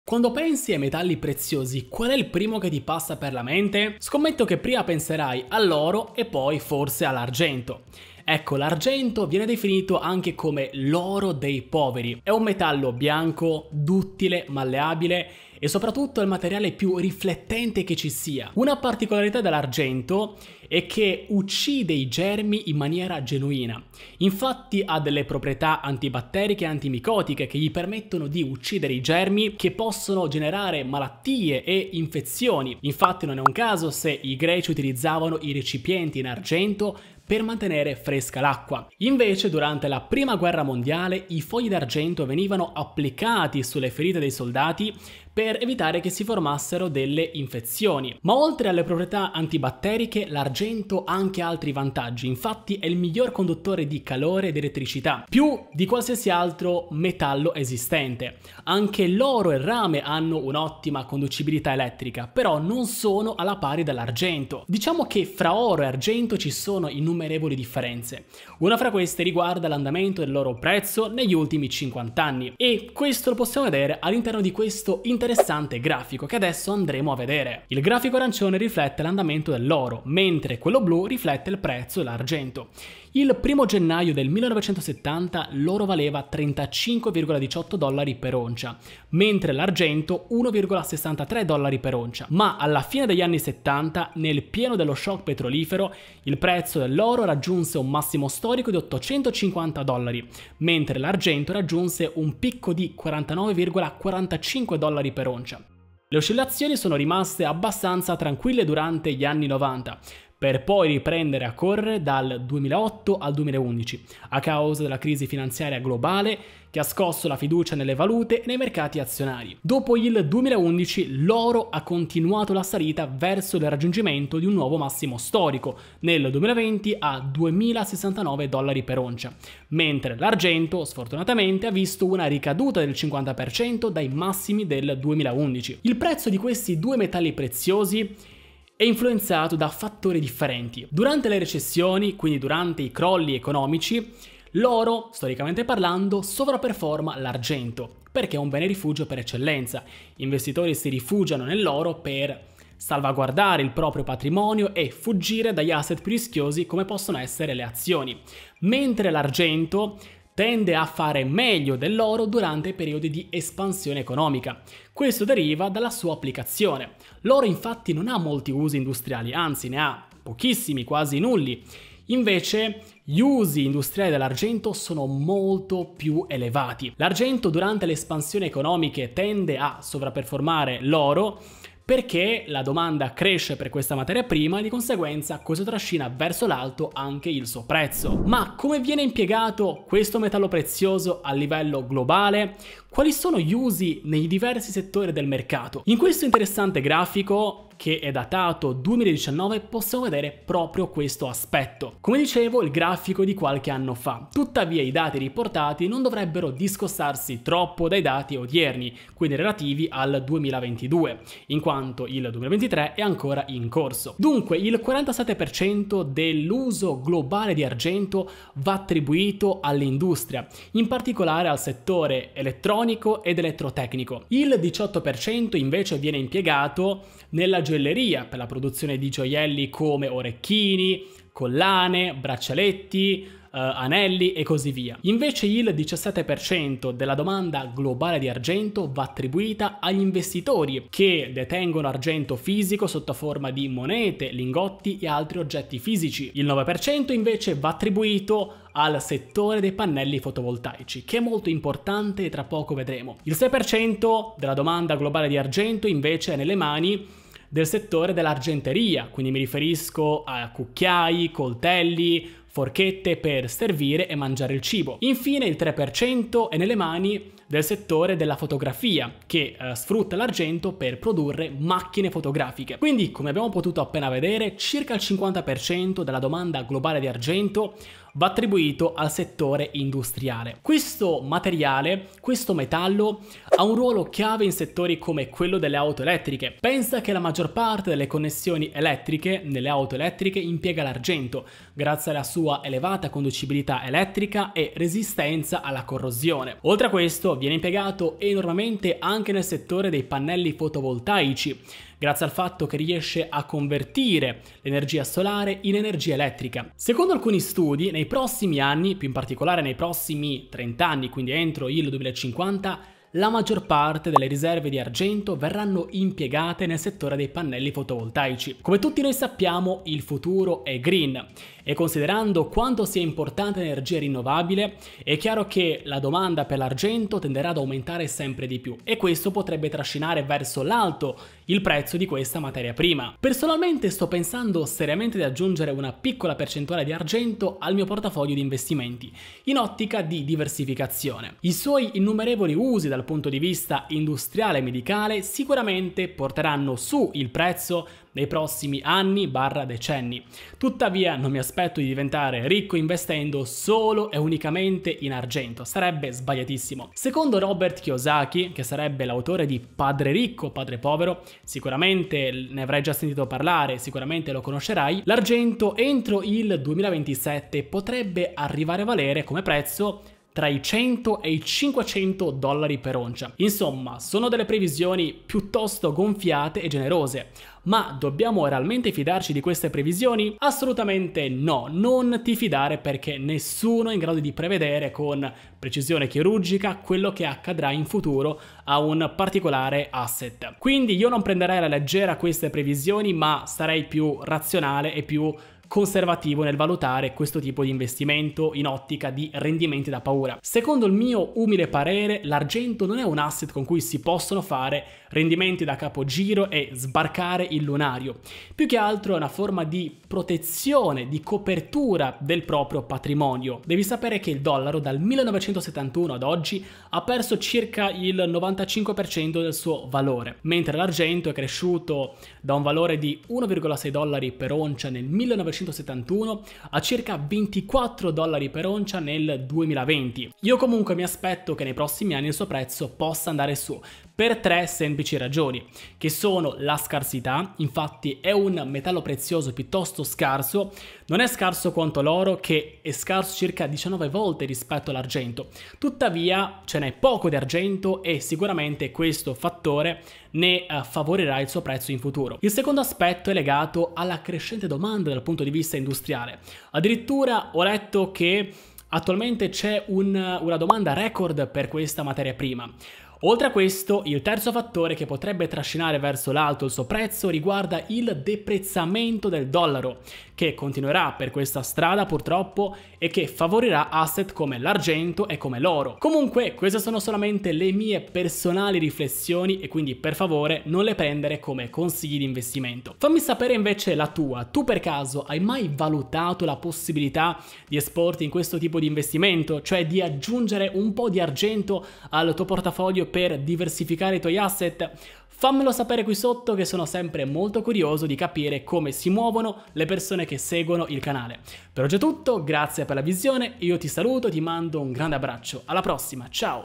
Prezzo Argento Potrebbe Esplodere. Ecco Perché
Alcune riflessioni sul futuro dell’Argento in ottica di investimento.

L’Argento è conosciuto come l’oro dei poveri. E’ un metallo bianco, malleabile, duttile ed è anche il materiale più riflettente che ci sia. Anche se Oro e Argento sono entrambi metalli preziosi, fra i due esistono molte differenze. Il loro prezzo è influenzato da fattori differenti. Durante i periodi di recessione, l’oro storicamente parlando sovraperforma l’argento. Mentre durante i periodi di espansione economica l’argento tende a fare meglio del metallo giallo. L’argento infatti ha molti usi industriali e durante l’espansione economica la sua domanda aumenta. Ad oggi circa il 50% della domanda è nelle mani dell’industria. In effetti questo metallo gioca un ruolo chiave in settori come quello delle auto elettriche e dell’energia solare. Alcuni studi affermano che la maggior parte delle riserve di Argento sulla terra potrebbero essere impiegate nei prossimi 30 anni nei pannelli fotovoltaici. Ma come si comporterà il suo prezzo in futuro? Arriverà ad esplodere così come dice Robert Kiyosaki?